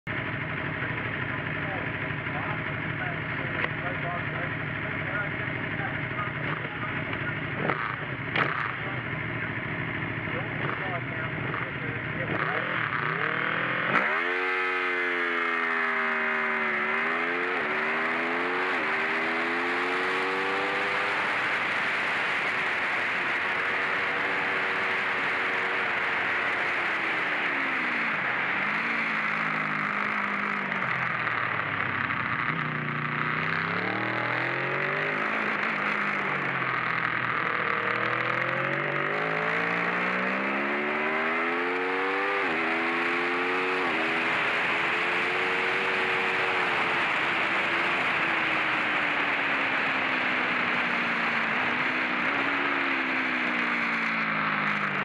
Tuono V4 at Gurston Down Hill Climb 17th June 2018
Another angle of the V4 up Gurston Down at the Tony Marsh memorial weekend.